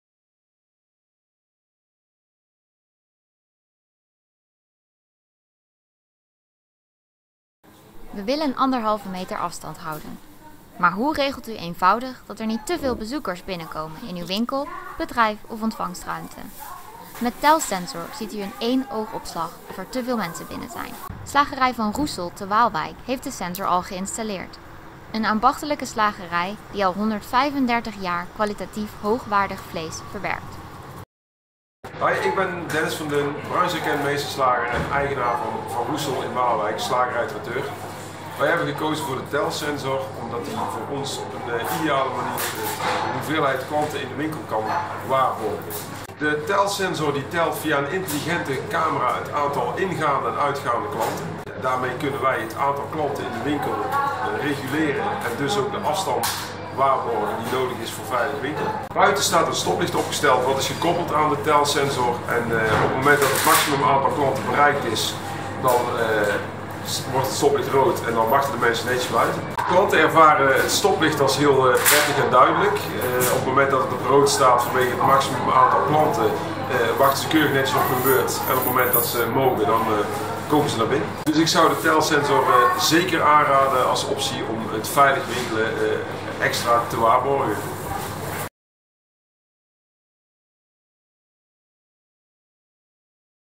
Vlog: VCS installeerde TelSensor bij slagerij-traiteur van Roessel
Social distancing waarborgen in retail is een uitdaging, de boetes zijn hoog. Daarom ontwikkelde VCS de praktische oplossing TelSensor. VCS installeerde TelSensor recent bij verschillende ondernemingen, waaronder slagerij-traiteur Van Roessel in Waalwijk. In de vlog ziet u hoe het werkt.